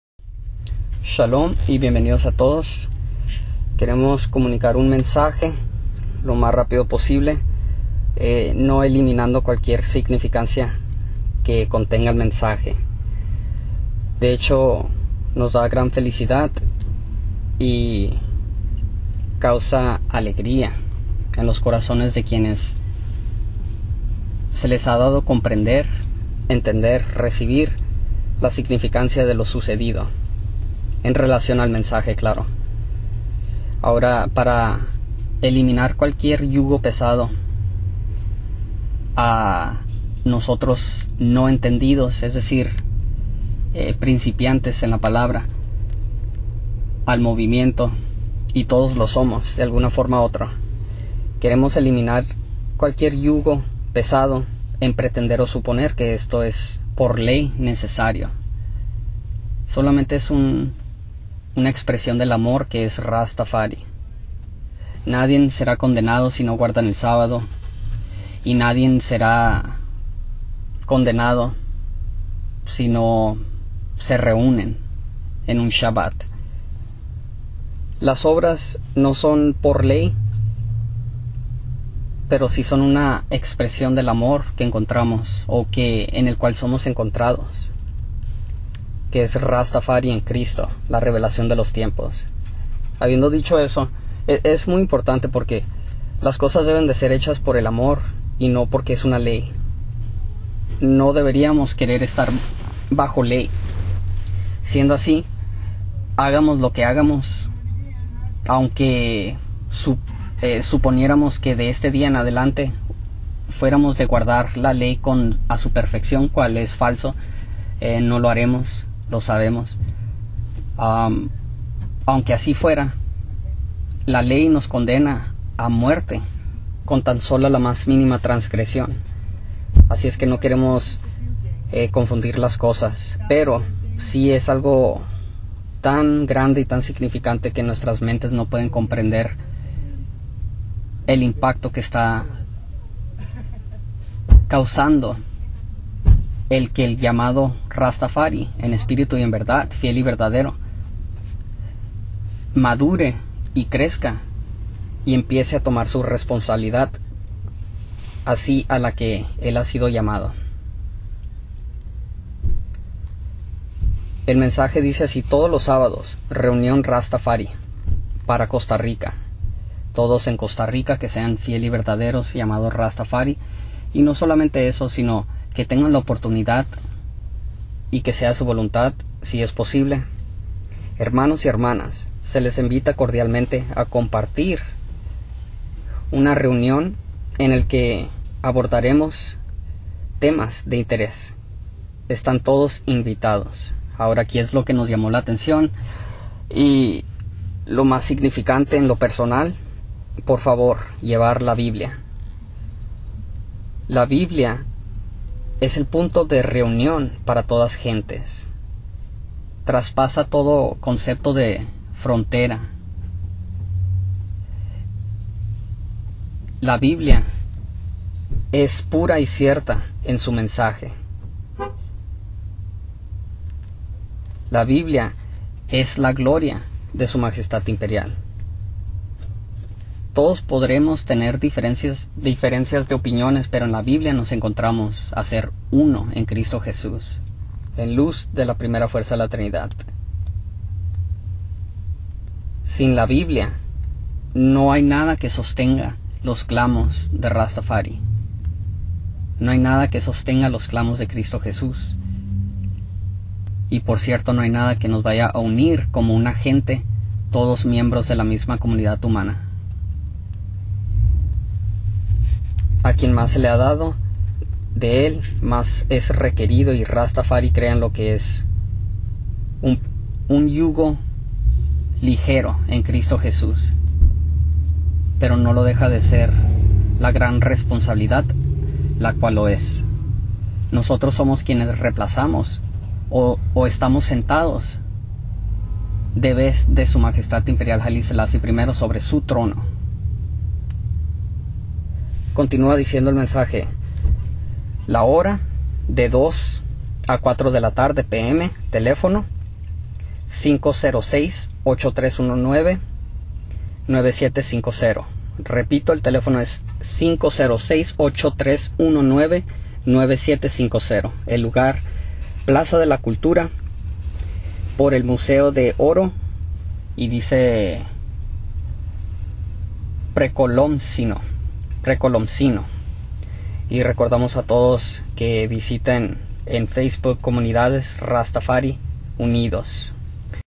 COMUNIDADES RASTAFARI UNIDOS-REUNION TODO SABADO EN COSTA RICA "lleven la BIBLIA"
LOJSocietyEspañol-"SOCIEDAD LEON DE JUDA
todos los sabados reunion RasTafari para Costa Rica; Hermanos y Hermanas se les invita cordialmente a compartir una reunion en el que abordaremos temas de interes, estan todos invitados; por favor, llevar la Biblia-Horario:2-4pm Telefono (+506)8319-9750_Lugar: Plaza de la Cultura, por el museo de oro precolombino (COMUNIDADES RASTAFARI UNIDOS en facebook)